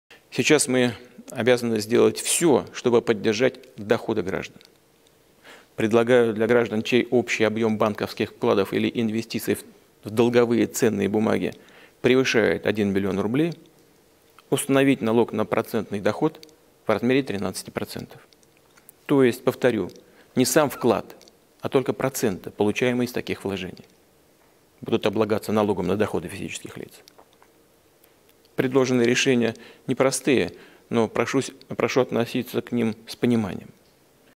Путин предлагает поддержать доходы граждан.
Обращение Путина к россиянам в связи с ситуацией по коронавирусу